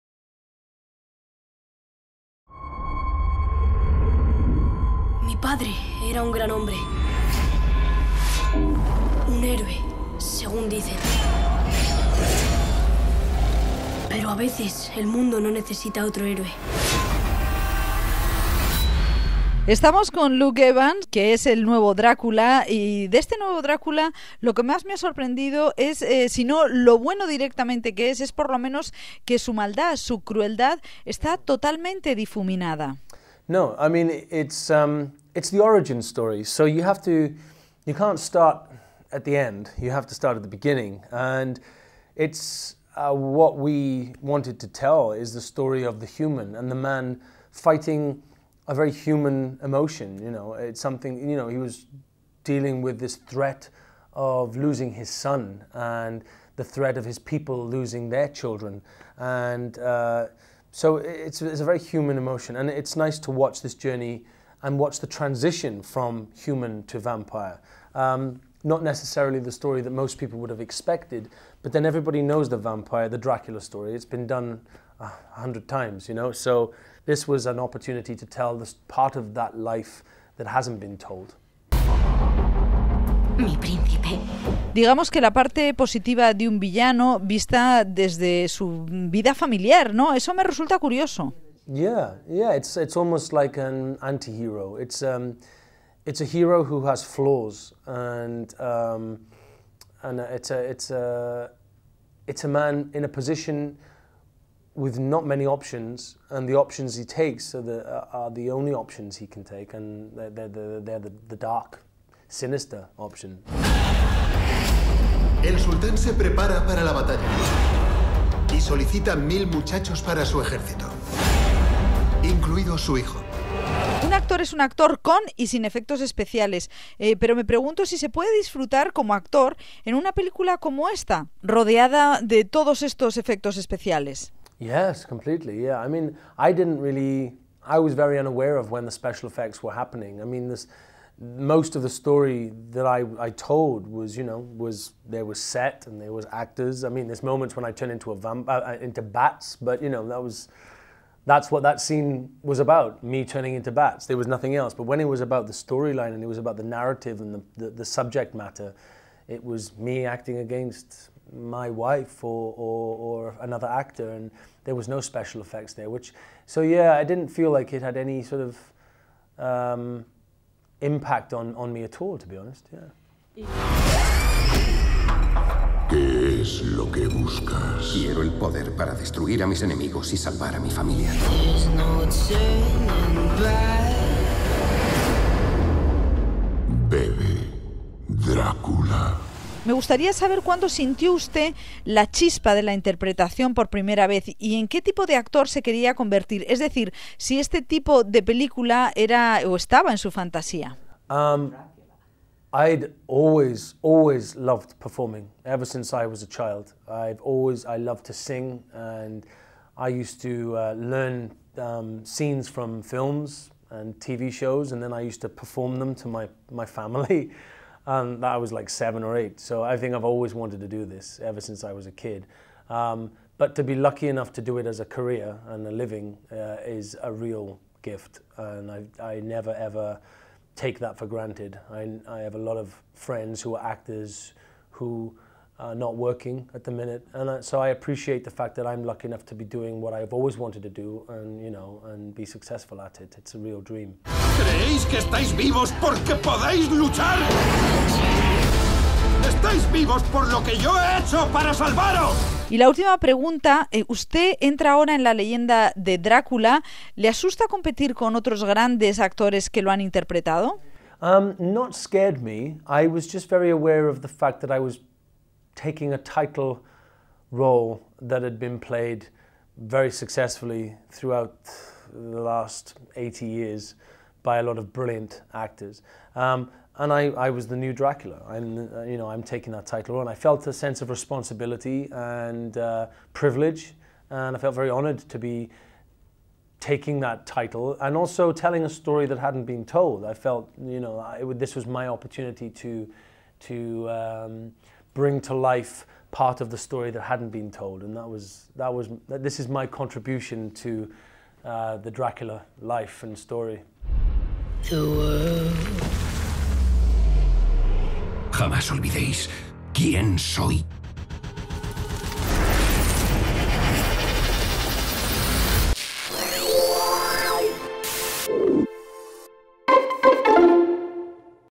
Luke Evans: "Mi Drácula es un antihéroe". Cadena SER
Entrevistamos al nuevo Drácula, el actor galés Luke Evans que protagoniza 'Drácula, la leyenda jampás contada'